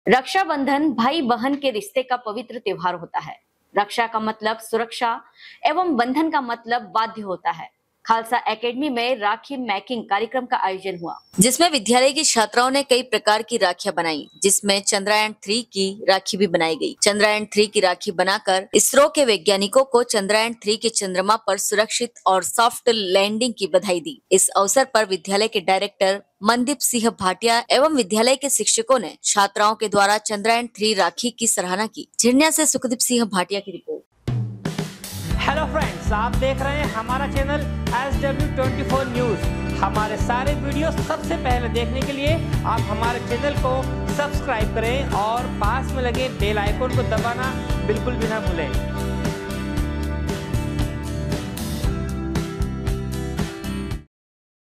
रक्षाबंधन भाई बहन के रिश्ते का पवित्र त्यौहार होता है, रक्षा का मतलब सुरक्षा एवं बंधन का मतलब बाध्य
रक्षाबंधन भाई बहन के रिश्ते का पवित्र त्यौहार होता है, रक्षा का मतलब सुरक्षा एवं बंधन का मतलब बाध्य होता है, खालसा एकेडमी में राखी मेकिंग कार्यक्रम का आयोजन हुआ
झिरन्या से सुखदिप भाटीया की रिपोर्ट।